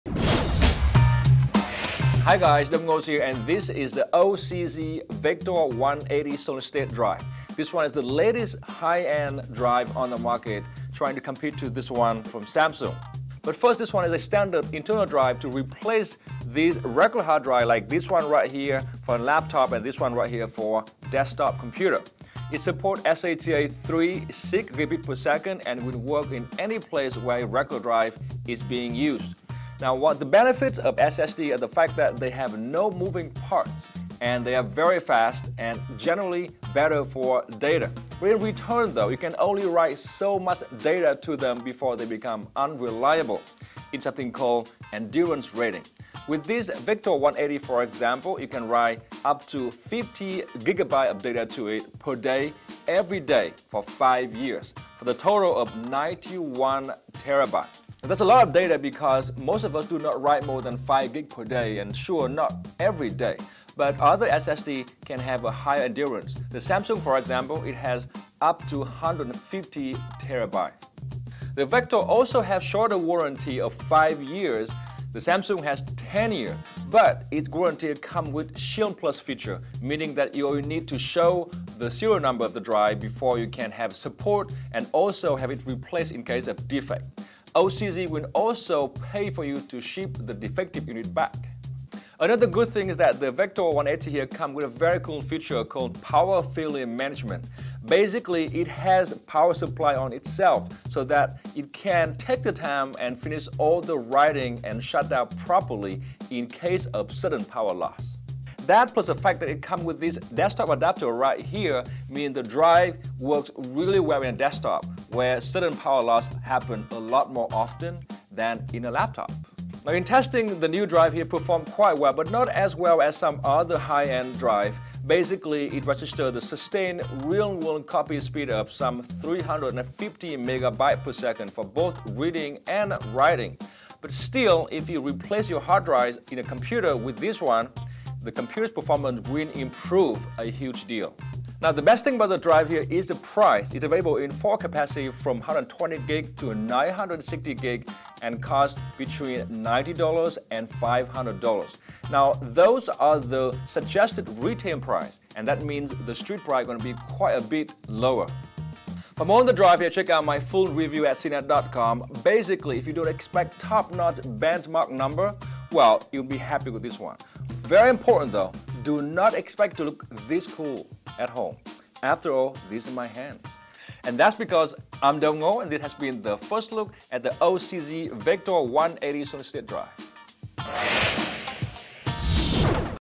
The OCZ Vector 180 is a good SSD for desktops
Been wondering why the SSD you have at home doesn't look as good as it does on TV? CNET editor Dong Ngo has the answer with the help of the OCZ Vector 180.